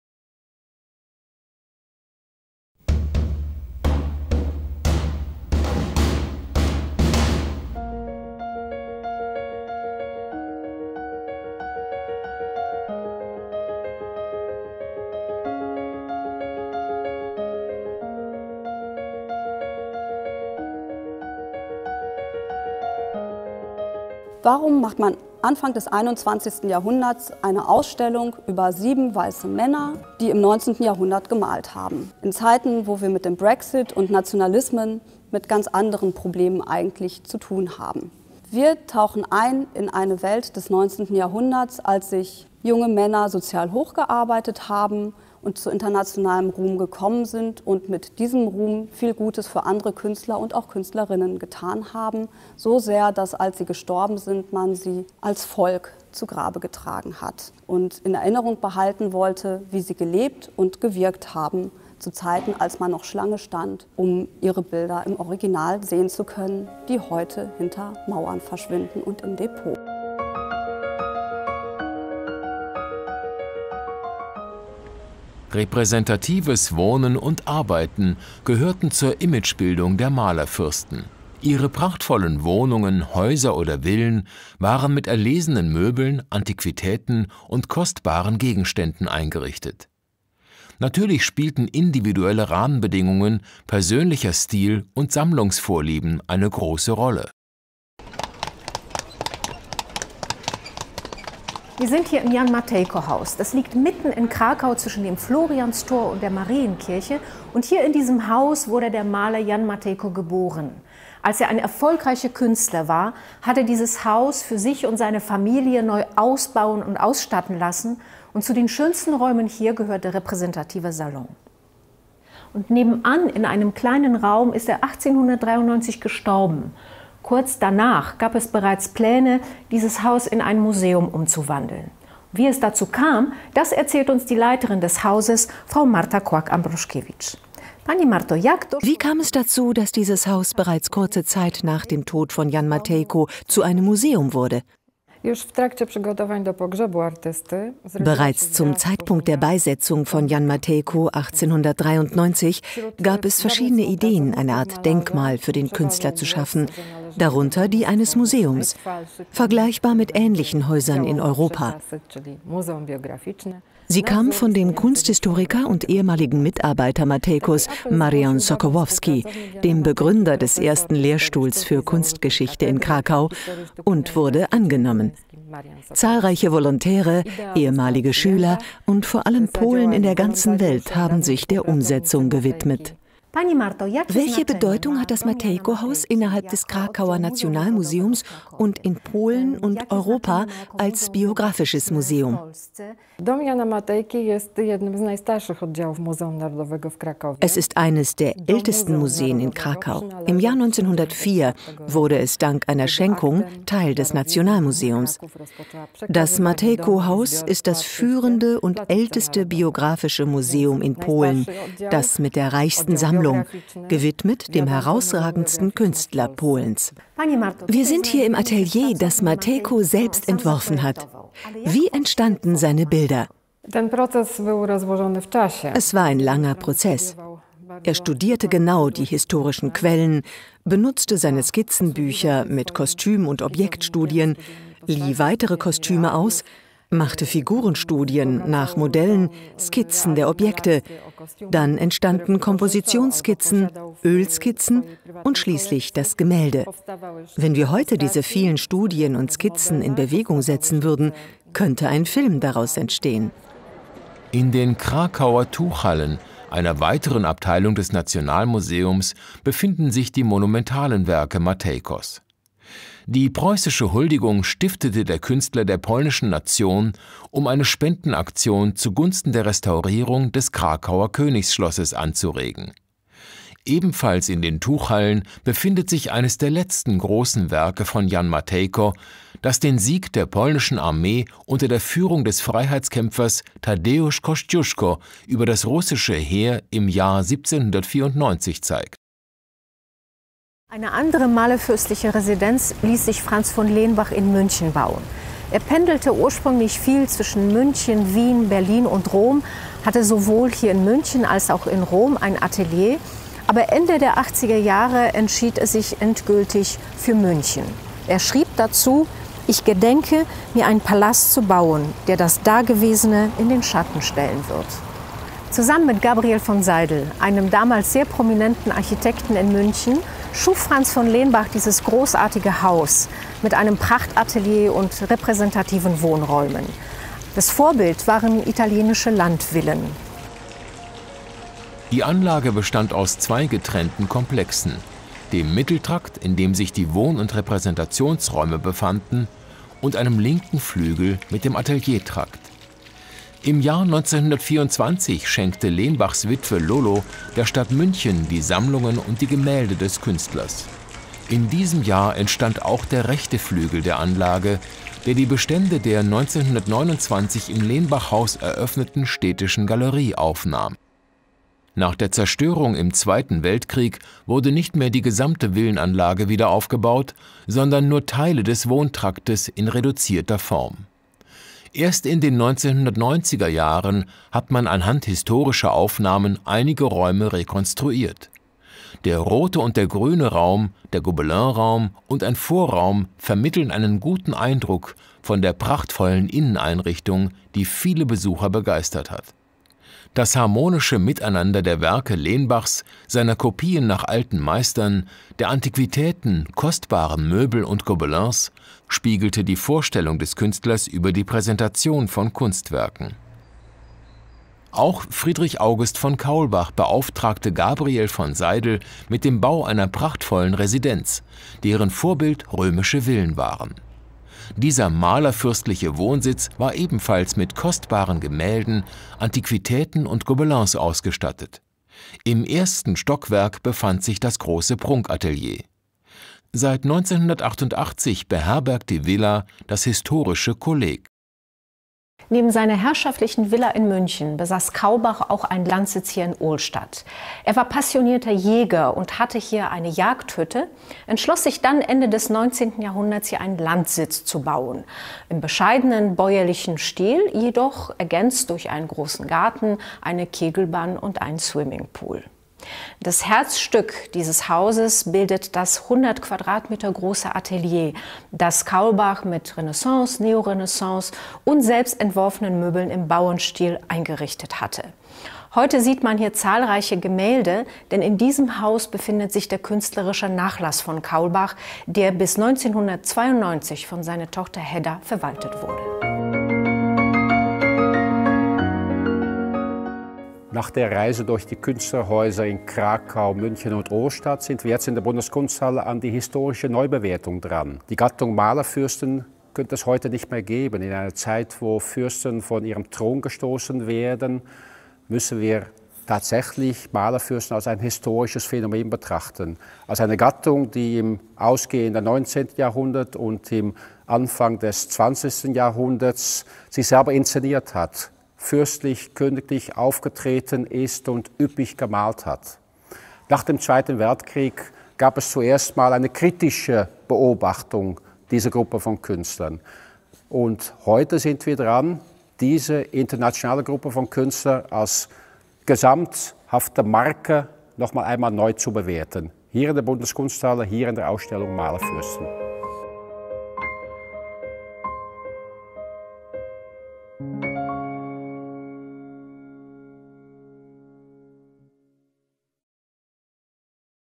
Malerfürsten – Behind The Art
Bereits in der Renaissance und im Barock etablierten Maler wie Raffael, Tizian, Rubens und van Dyck ihren gesellschaftlich anerkannten und höfisch privilegierten Sonderstatus. Als Malerfürsten zeigten sie sich in ihrem Verhalten und in ihren Selbstbildnissen. Ausgehend von diesen historischen Vorbildern widmet sich die kunst- und kulturhistorische Ausstellung dem länder- und epochenübergreifenden Phänomen der Malerfürsten. Sie stellt ihre Blütezeit im 19. Jahrhundert anhand der Beispiele von Frederic Lord Leighton, Hans Makart, Franz von Lenbach, Mihály Munkácsy, Jan Matejko, Friedrich August von Kaulbach und Franz von Stuck in den Mittelpunkt.

Im Zentrum der Ausstellung stehen die Selbst- und Fremdinszenierungen der modernen Malerfürsten und der sich um sie rankende Künstlerkult. Erfolgreich nutzten sie die Macht ihrer Netzwerke und ihre Popularität für den sozialen Aufstieg. Durch neue Reproduktionsmedien, Ausstellungsinszenierungen, Atelierbesuche und Interviews in Tageszeitungen wurden die Künstler und ihre Werke weltweit mit großem Gewinn vermarktet und das gesellschaftliche Konzept „Malerfürst“ erfolgreich etabliert.

MALERFÜRSTEN
28. September 2018 bis 27. Januar 2019
in der Bundeskunsthalle, Bonn

In Kooperation mit Muzeum Narodowe w Krakowie (Nationalmuseum in Krakau).